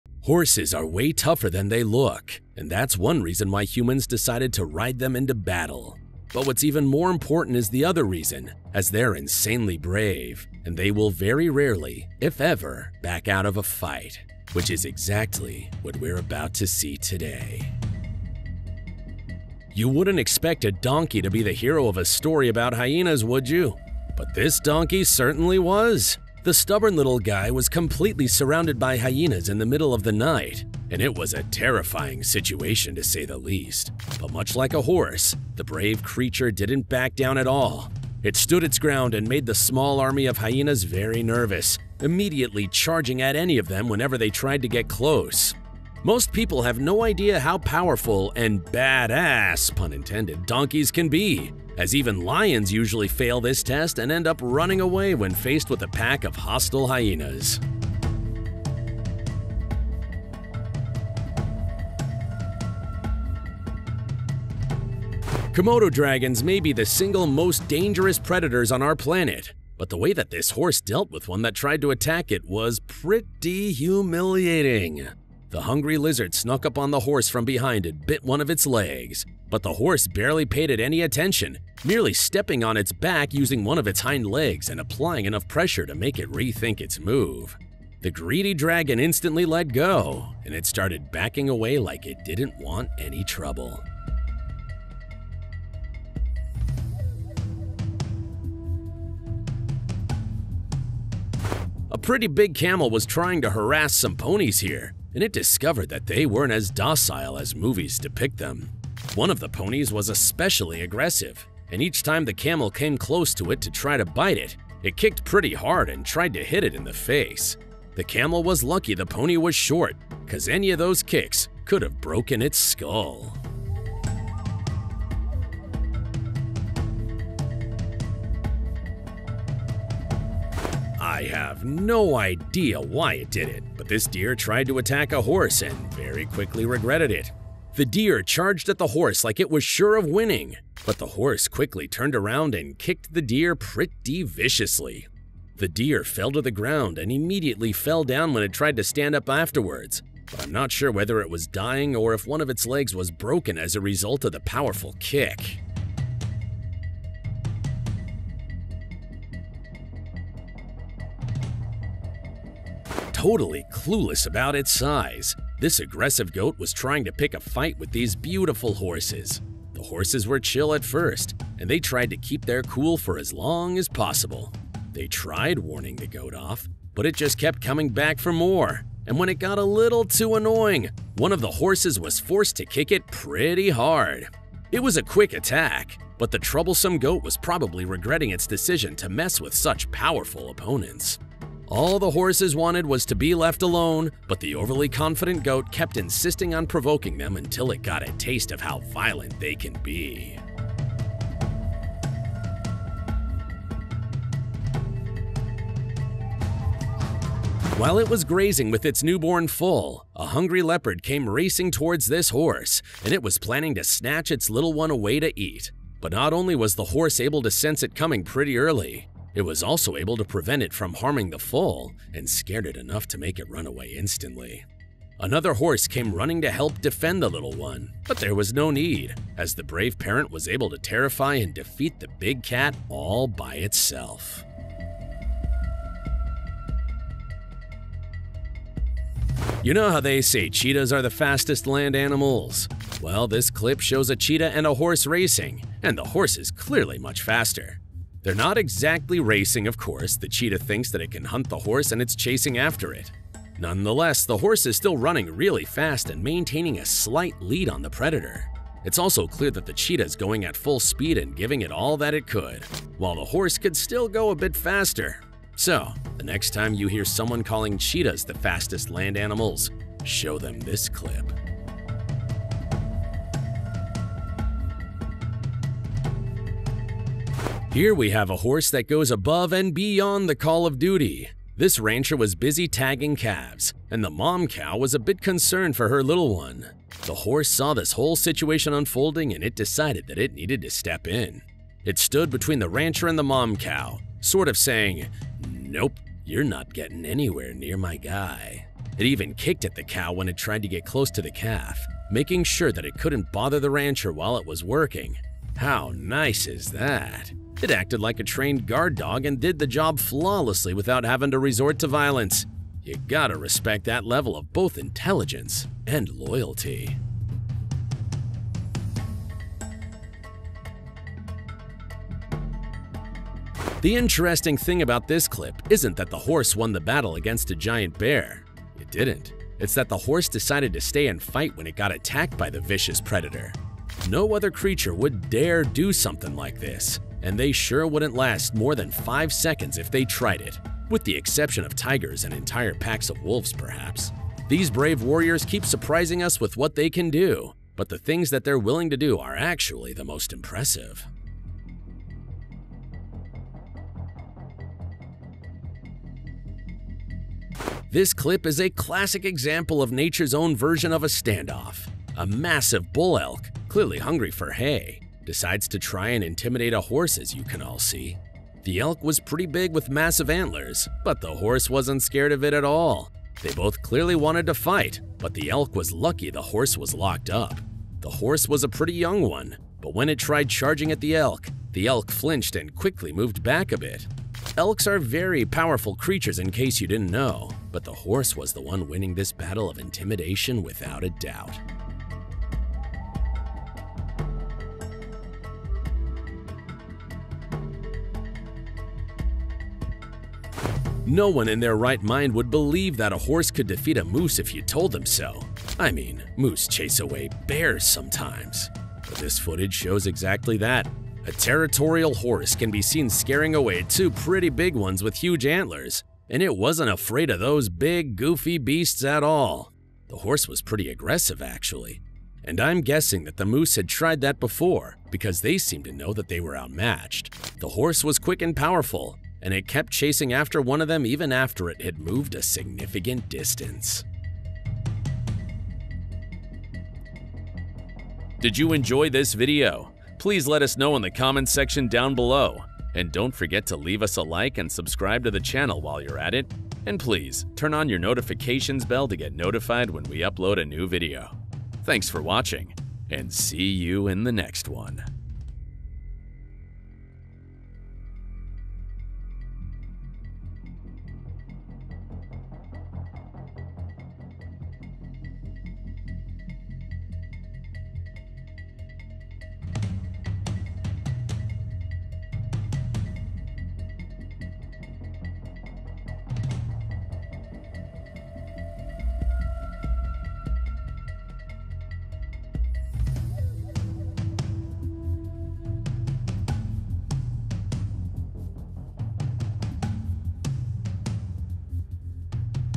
Look What Happened When This Leopard Attacked Horse
Horses are way tougher than they look, and that’s one reason why humans decided to ride them into battle.